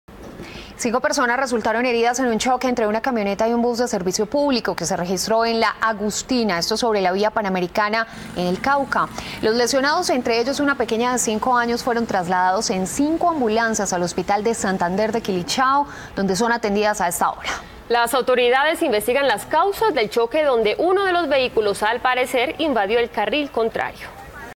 Aparatoso accidente de tránsito en vía Panamericana deja 5 heridos en Cauca
Hasta el hospital de Santander de Quilichao fueron remitidas cinco personas, queresultaron heridas tras choque entre una buseta de servicio intermunicipal y una camioneta particular. El hecho se registró hacia las 5:30 p.m. de este domingo, en La Agustina, sobre la vía Panamericana, Cauca.Entre los lesionados se encuentran una menor de 5 años y otra de 16 años. Autoridades investigan las causas de la colisión, donde uno de los vehículos, al parecer, invadió el carril contrario.